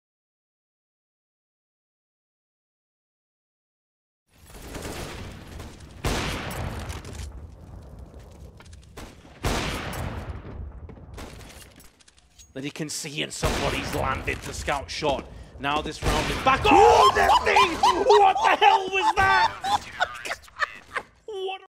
Nifty three AWP wallbangs vs. TyLoo on Cache
The Australian player Noah "Nifty" Francis gets three insane wallbangs with the AWP in the PGL Major Krakow 2017-quialifying match against TyLoo.